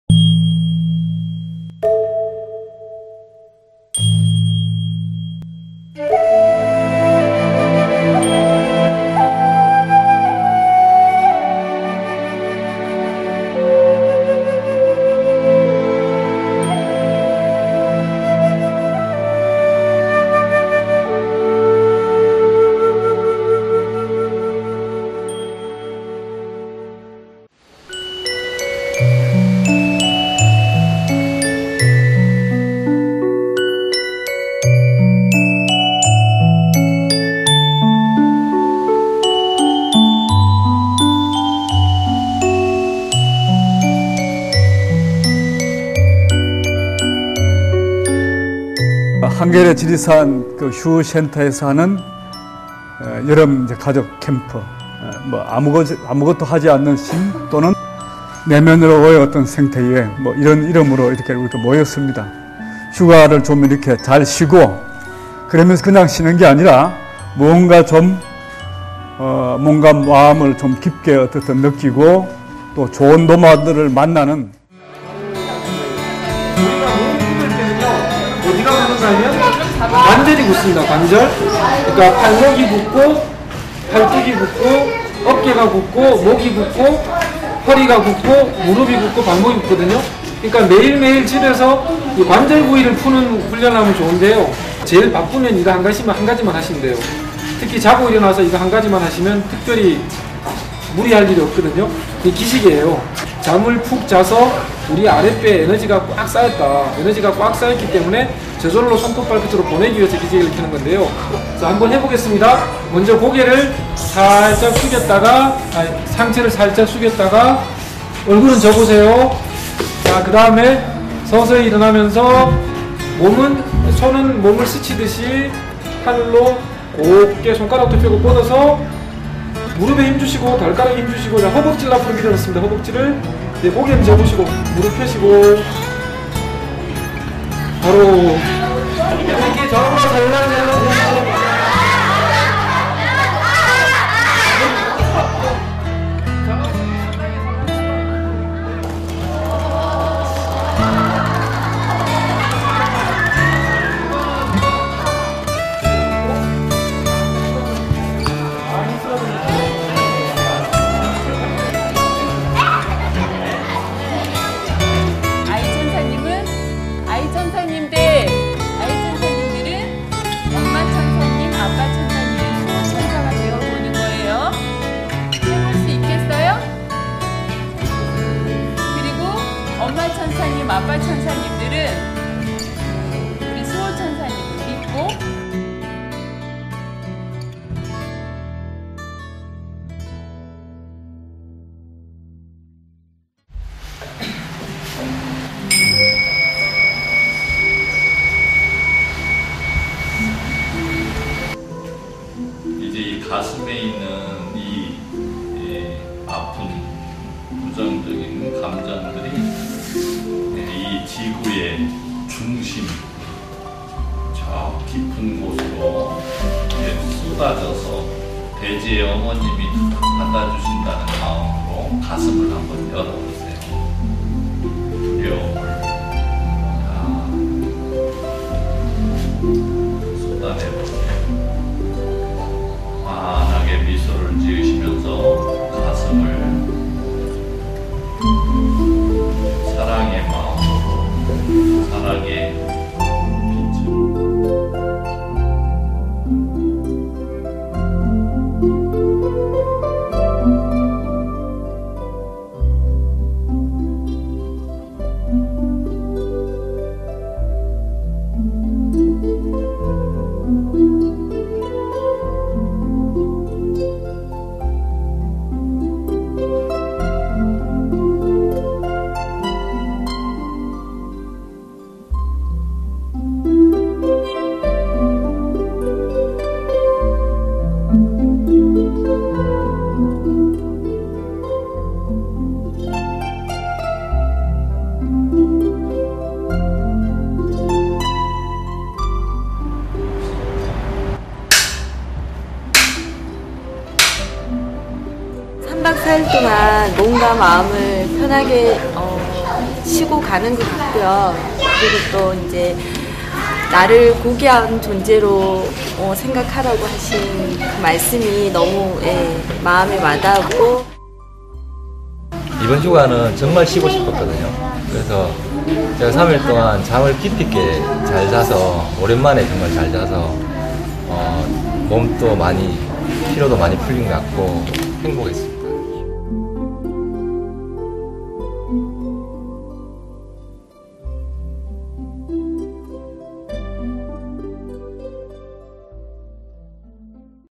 한겨레 '휴' 지리산센터...아무 것도 하지 않는 행복
가족과 함께 지리산에서 온전한 휴식 3박4일
가족 기살리기체조·마인드힐링명상·부부명상